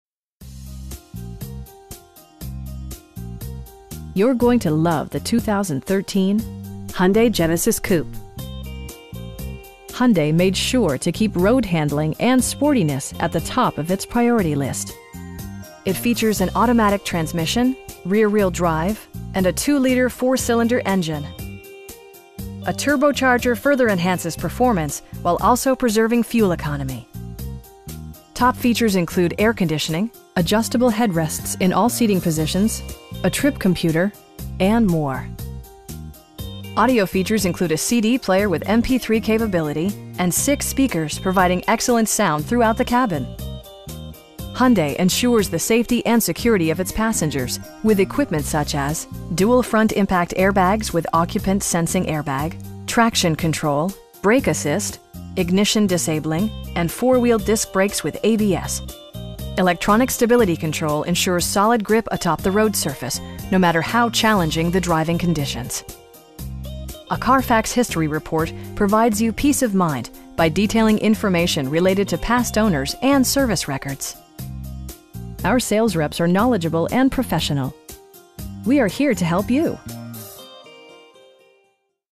2013 Hyundai Genesis Coupe 2.0T in Allentown, PA 18103
Ciocca Subaru
4611 Hamilton Blvd

You're going to love the 2013 Hyundai Genesis Coupe. Hyundai made sure to keep road-handling and sportiness at the top of it's priority list. It features an automatic transmission, rear-wheel drive, and a 2 liter 4 cylinder engine. A turbocharger further enhances performance, while also preserving fuel economy. Top features include air conditioning, delay-off headlights, 1-touch window functionality, adjustable headrests in all seating positions, a trip computer, front bucket seats, tilt steering wheel, and more. Audio features include a CD player with MP3 capability, and 6 speakers, providing excellent sound throughout the cabin. Hyundai ensures the safety and security of its passengers with equipment such as: dual front impact airbags with occupant sensing airbag, head curtain airbags, traction control, brake assist, anti-whiplash front head restraints, ignition disabling, and 4 wheel disc brakes with ABS. Electronic stability control ensures solid grip atop the road surface, no matter how challenging the driving conditions. A Carfax history report provides you peace of mind by detailing information related to past owners and service records. Our sales reps are knowledgeable and professional. We'd be happy to answer any questions that you may have. We are here to help you.